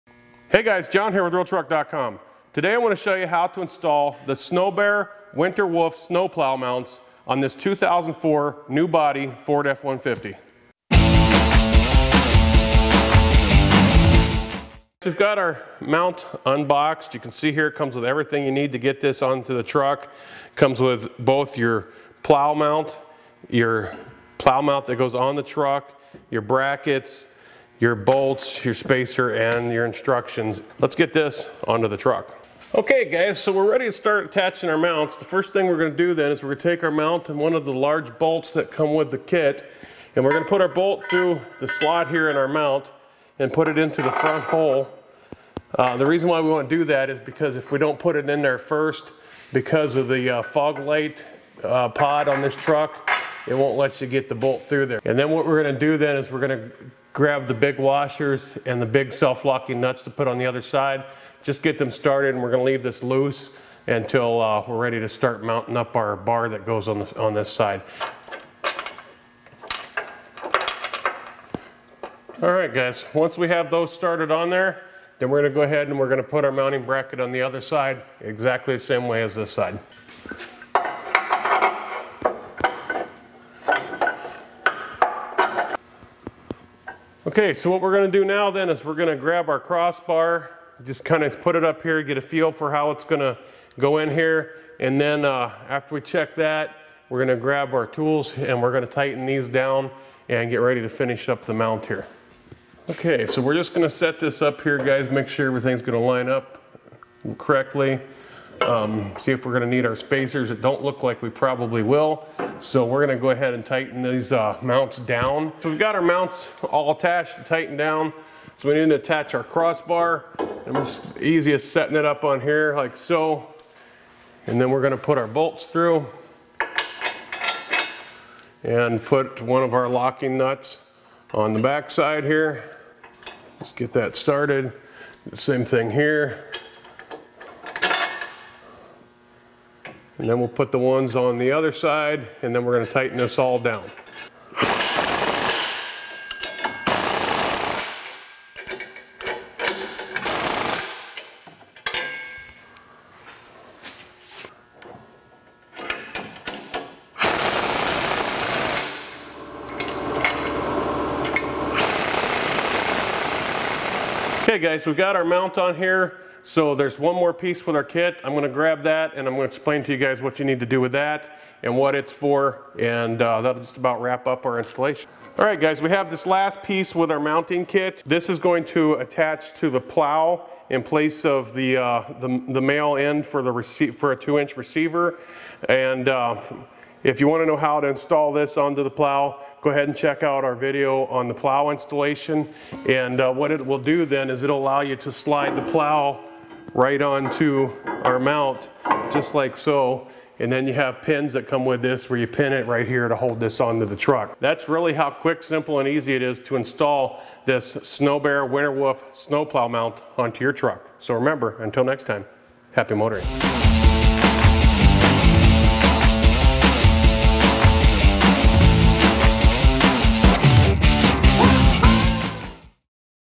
How to Install SnowBear WinterWolf Snow Plow Mounts
The SnowBear WinterWolf Snow Plow Mounts are custom to your vehicle to give you the best fit. Easy bolt-on installation that can be done right at home. Designed to fit all WinterWolf snow plows. Black powder-coated finish that is resistant to rust.

Video Transcription:
Hey guys, John here with RealTruck.com. Today I want to show you how to install the Snowbear Winterwolf Snow Plow Mount on this 2004, new body, Ford F150.

[music]

We've got our mount unboxed, you can see here it comes with everything you need to get this onto the truck. It comes with both your plow mount, your plow mount that goes on the truck. Your brackets, your bolts, your spacer, and your instructions. Let's get this onto the truck.

OK, guys. So we're ready to start attaching our mounts. The first thing we're going to do is we're going to take our mount and one of the large bolts that come with the kit and we're going to put our bolt through the slot in our mount and put it into the front hole. The reason why we want to do that is because if we don't put it in there first, because of the fog light pod on this truck, it won't let you get the bolt through there. And then what we're going to do then, is we're going to grab the big washers and the big self-locking nuts to put on the other side, just get them started and we're going to leave this loose until we're ready to start mounting up our bar that goes on this side.

Alright, guys. Once we have those started on there, then we're going to go ahead and put our mounting bracket on the other side, exactly the same way as this side.

OK. So what we're going to do now then is to grab our crossbar, just kind of put it up here and get a feel for how it's going to go in here. And then, after we check that, we're going to grab our tools and we're going to tighten these down and we're going to finish up the mount here.

OK. So we're just going to set this up here and make sure everything's going to line up correctly, see if we're going to need our spacers. It don't look like we probably will, so we're going to go ahead and tighten these mounts down.

So we've got our mounts all attached and tightened down, so we need to attach our crossbar. It's easy as setting it up on here, like so. And then we're going to put our bolts through and put one of our locking nuts on the backside here. Get that started. Same thing here. And then we'll put the ones on the other side. And then we're going to tighten this all down.

OK, guys. We've got our mount on here, so there's one more piece with our kit, I'm going to grab that and explain to you guys what you need to do with that and what it's for and that will about wrap up our installation.